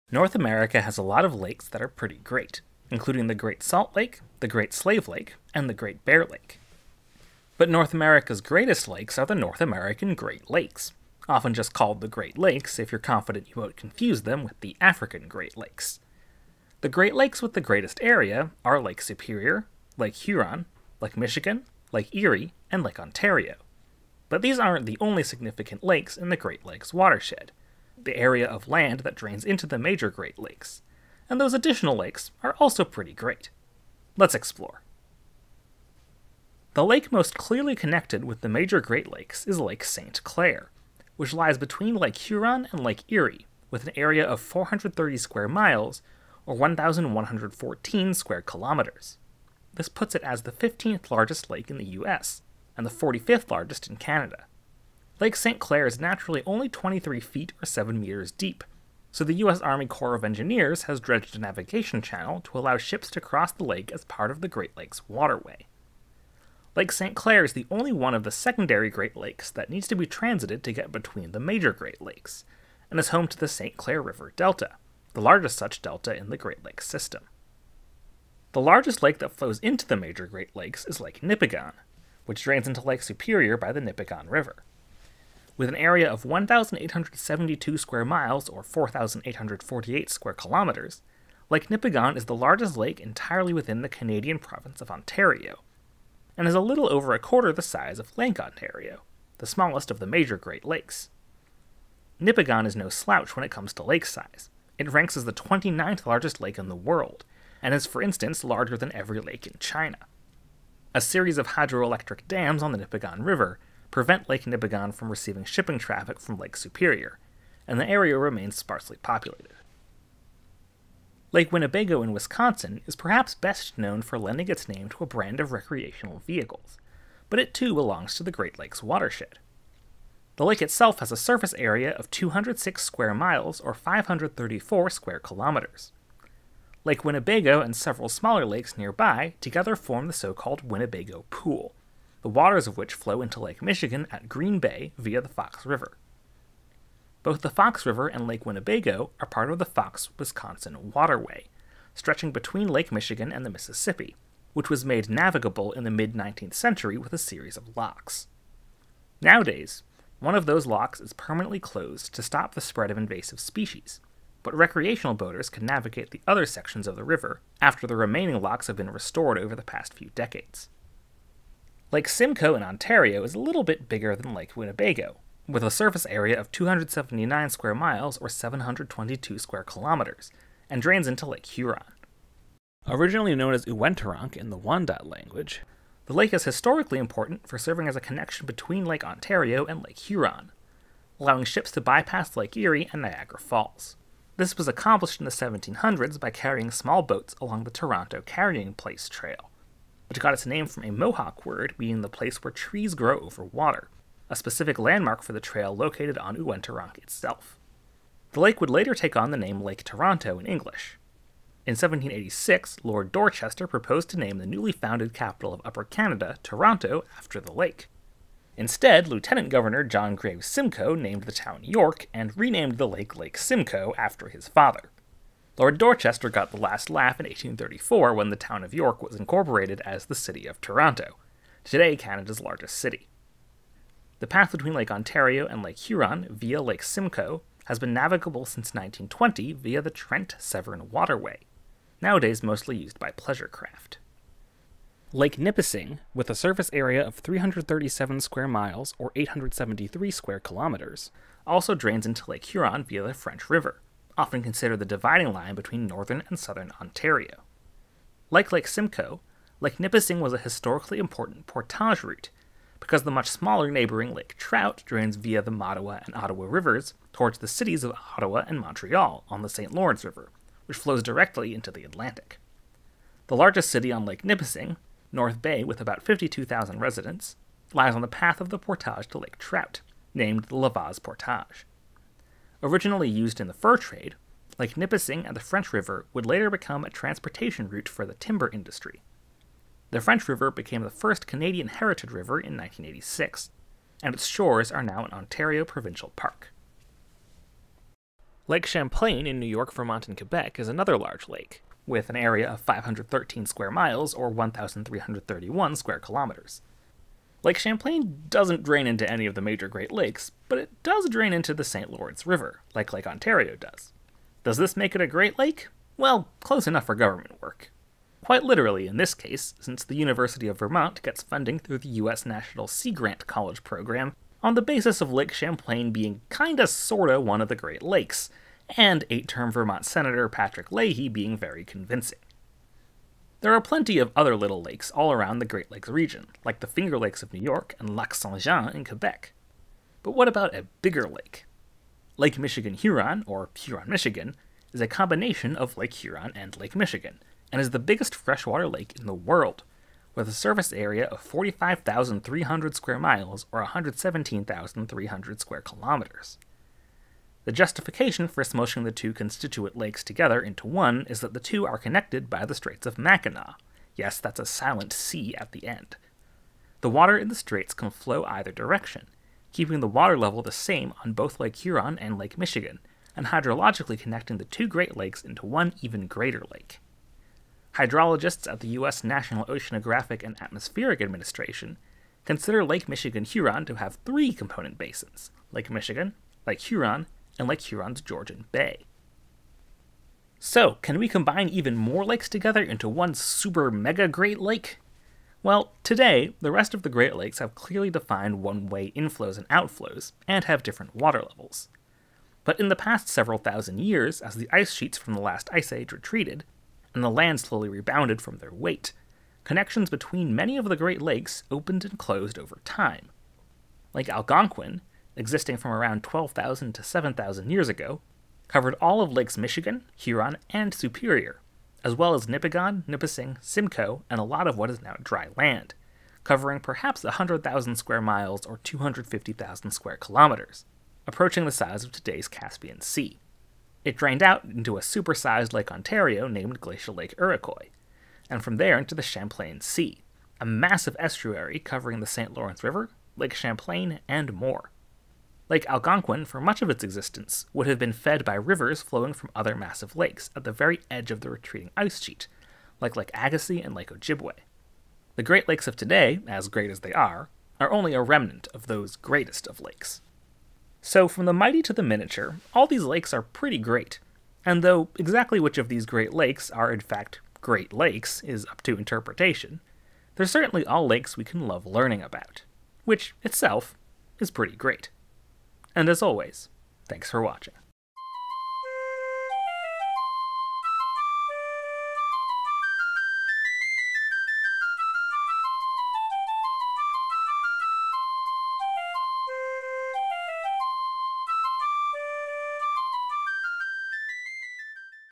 North America's Forgotten Great Lakes
North America has a lot of lakes that are pretty great, including the Great Salt lake, the Great Slave Lake, and the Great Bear Lake.  But North America’s greatest lakes are the North American Great Lakes, often just called the Great Lakes if you’re confident you won’t confuse them with the African Great Lakes.  The Great Lakes with the greatest area are Lake Superior, Lake Huron, Lake Michigan, Lake Erie, and Lake Ontario.  But these aren’t the only significant lakes in the Great Lakes watershed, the area of land that drains into the major Great Lakes, and those additional lakes are also pretty great.  Let’s Explore!

Chapters:
0:00 Intro
0:36 Lake St. Clair
1:18 Lake Nipigon
2:00 Lake Winnebago
2:51 Lake Simcoe
4:04 Lake Nipissing
4:57 Lake Champlain
5:32 Lake Michigan-Huron
6:30 Lake Algonquin
7:40 Conclusion

CORRECTIONS:
Nipigon is pronounced more like "Nipigin" or "Nipigen" (/ˈnɪpɪɡən/ for all you IPA fans)
"Portage" rhymes with "frontage", not "massage" (although some Canadians have mentioned they pronounce it the way I do in the video)
The lake next to Lake Nipissing is "Trout Lake", not "Lake Trout" which is a type of fish
The RV manufacturer Winnebago Industries is named after Winnebago County, Iowa, not Lake Winnebago.  Both names are related to the term "Winnebago" used to refer to the Ho-Chunk tribe.
The map showing Northern vs Southern Ontario isn't completely accurate, as some districts south of the French River are considered part of Northern Ontario as well.